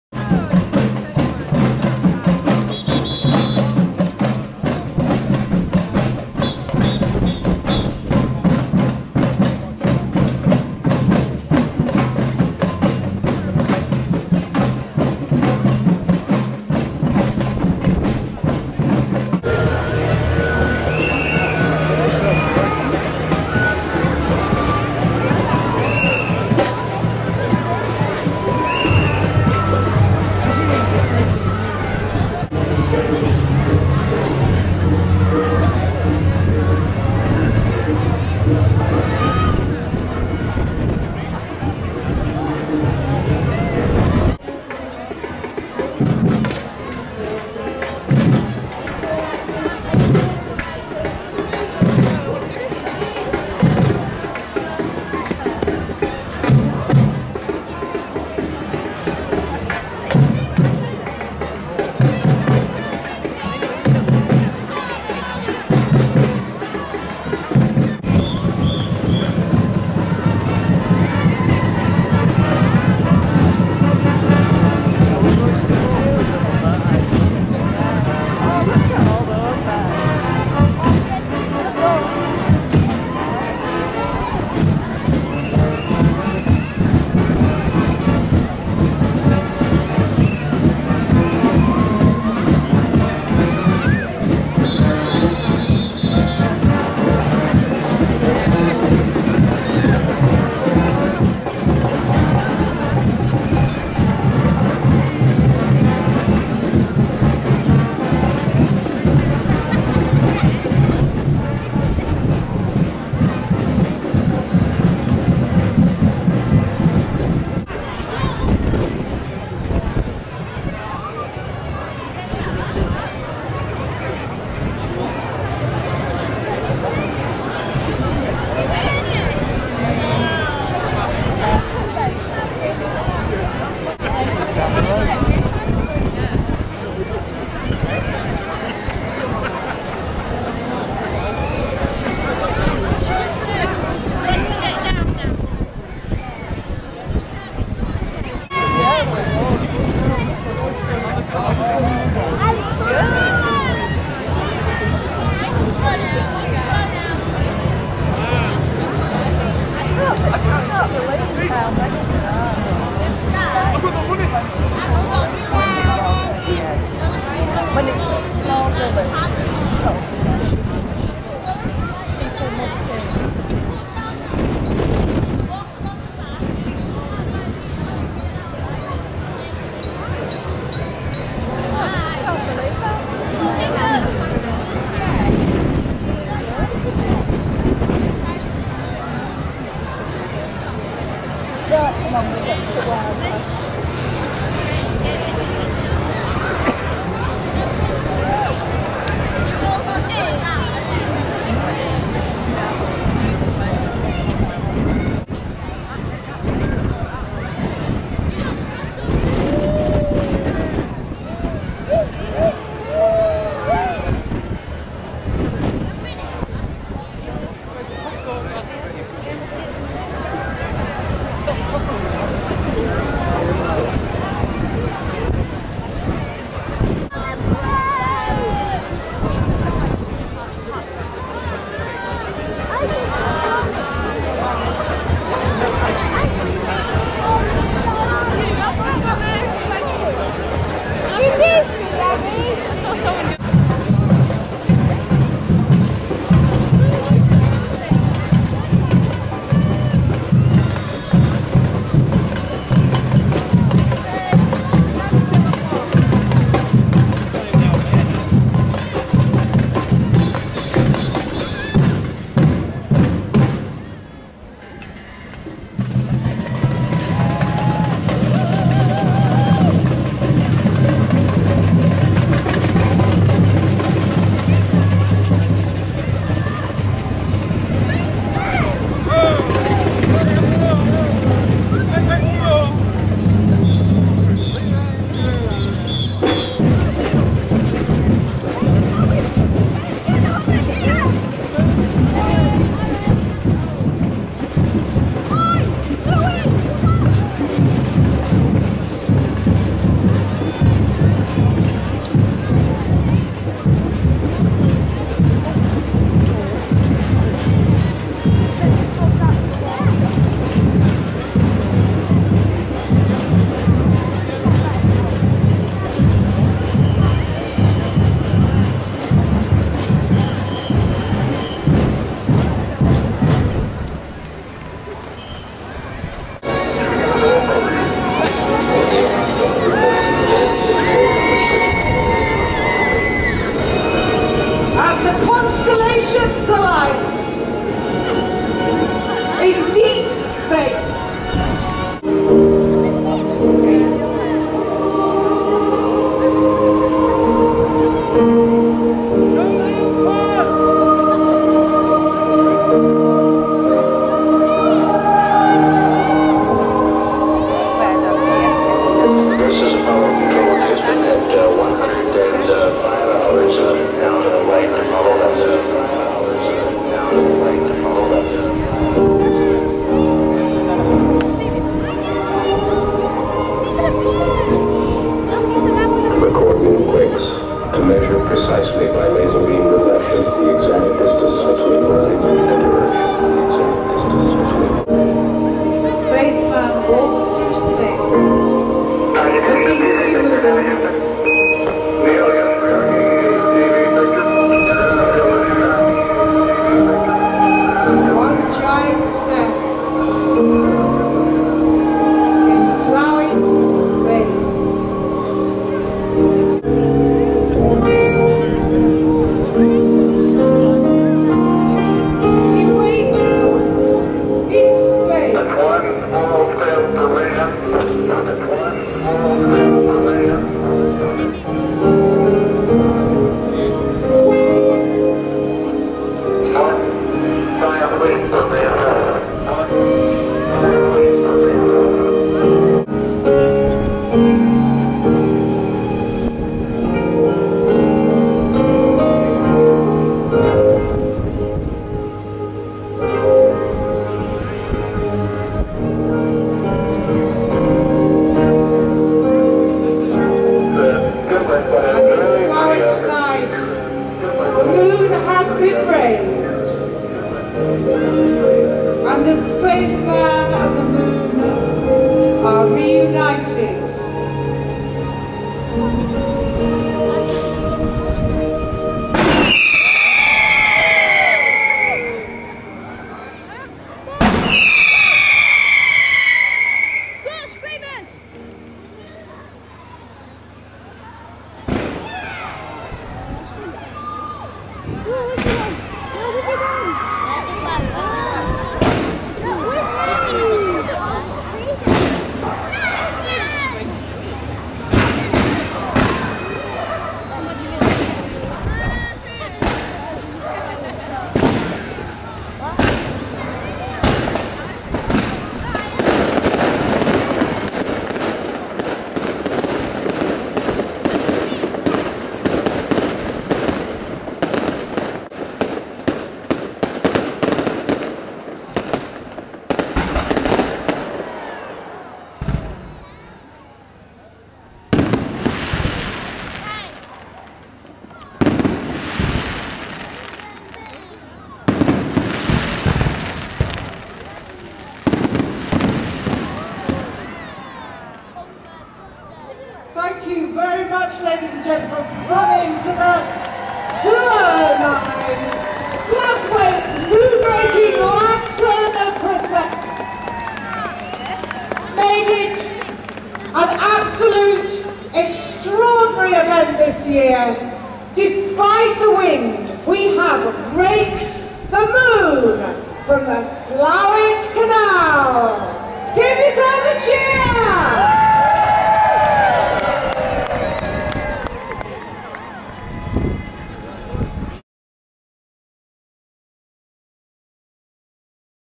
Slaithwaite Moonraking
The 19th century legend tells of smugglers bringing barrels of illicit drink by boat along the Huddersfield Narrow Canal at Slaithwaite when they saw the Constable up ahead.  To avoid arrest they pushed the barrels over the side of the boat.  Later that evening they returned under a full moon to retrieve the barrels but were discovered once again by the Constable as they were raking them in.  They escaped arrest by pretending to be drunk, saying that they were attempting to rake in the moon which had 'fallen into the canal', the moon being clearly visible in the water!  The lantern procession on the Saturday is the finale of a week of lanterns and legends with the theme for 2009 of 'Lunar Landings"